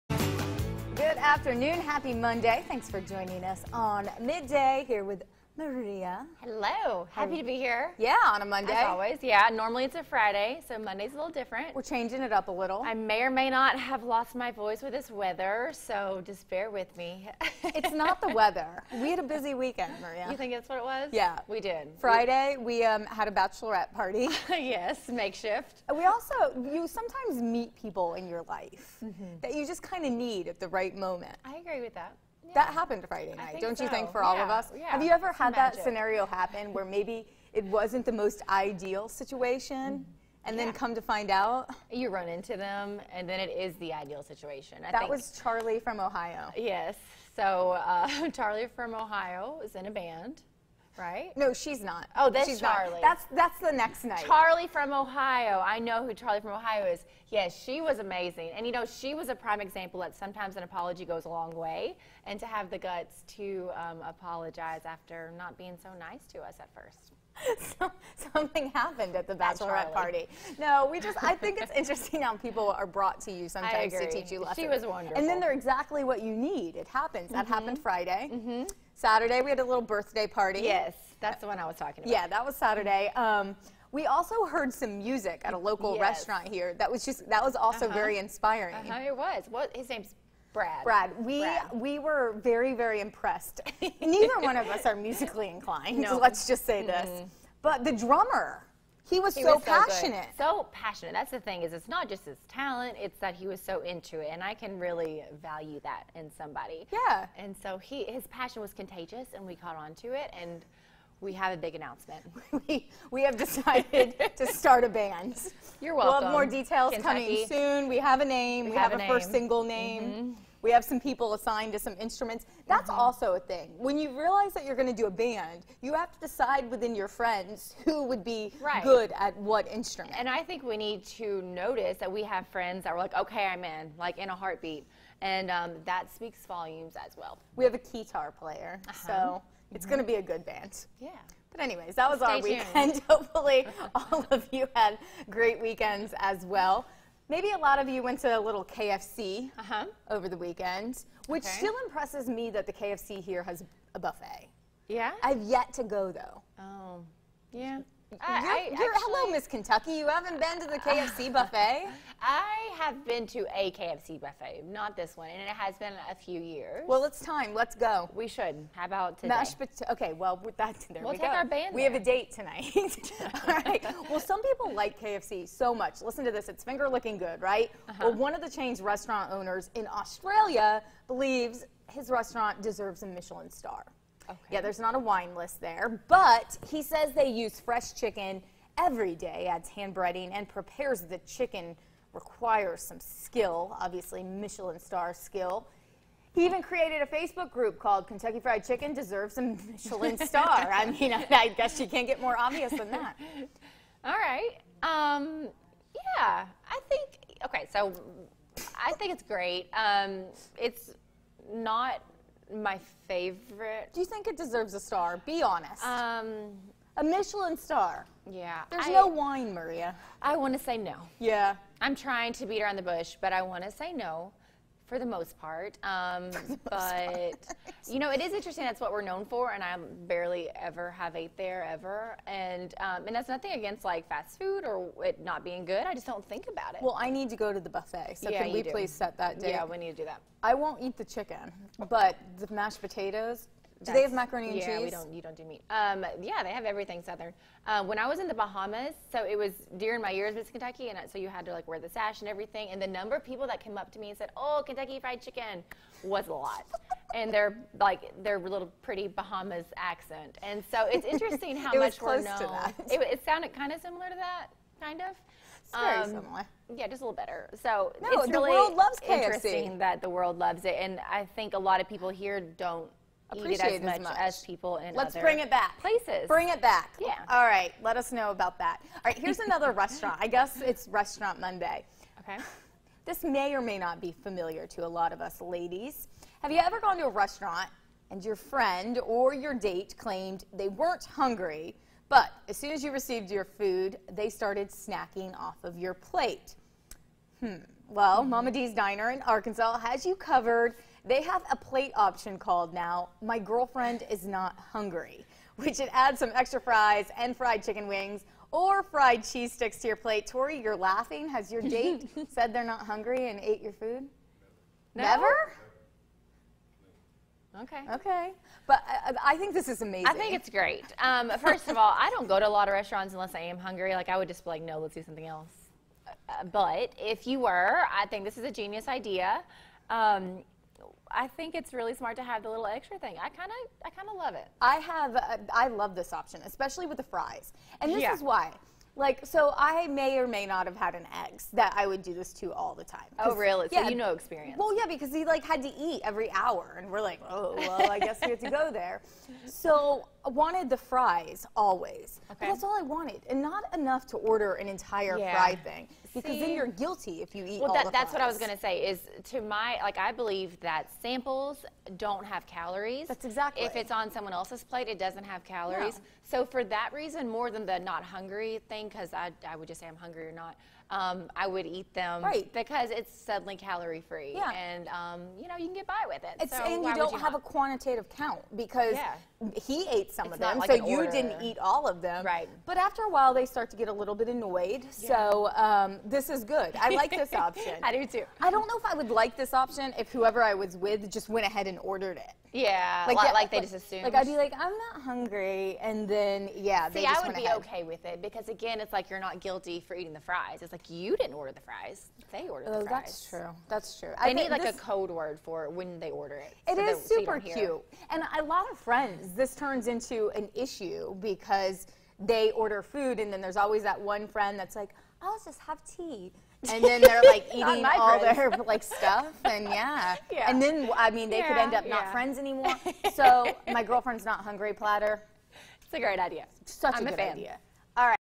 KFC Michelin star, quirky menu option, and more in Table Talk
Today’s headlines in Table Talk:
Some people believe KFC is "finger lickin' good", and one of the chain's restaurant owners in Australia believes they deserve a Michelin star.
Have you ever gone to a restaurant and your friend or date claimed they weren't hungry, but as soon as you received your food they started snacking off your plate?